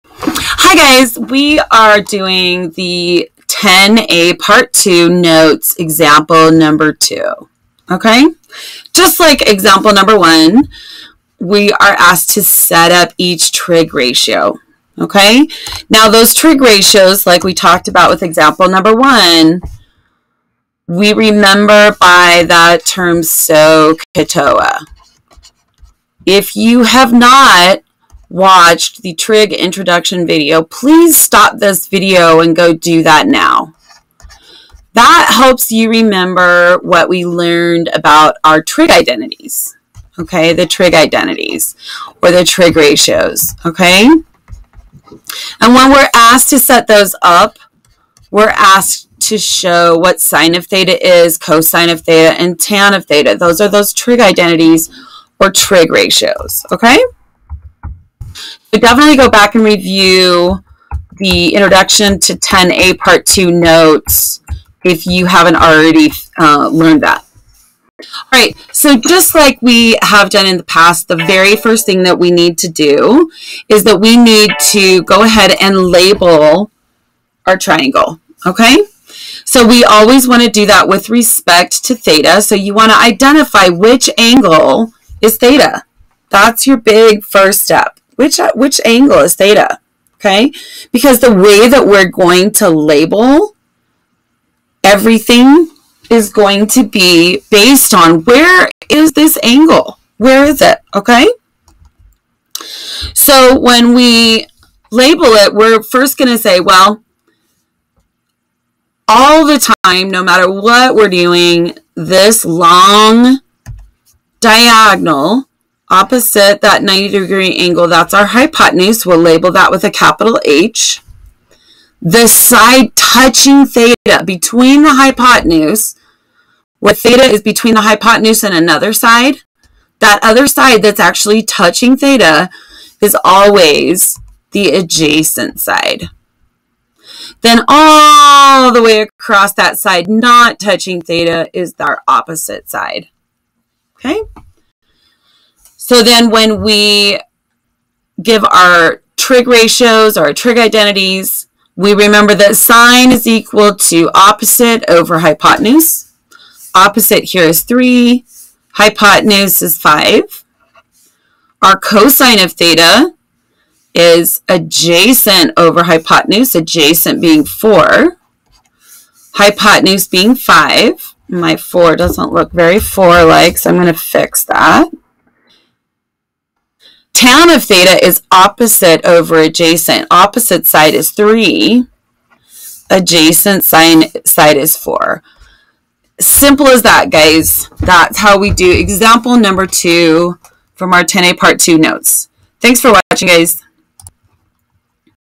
10A(2) Notes - Example #2
Intro to Trig - Setting up trig ratios from a labeled right triangle.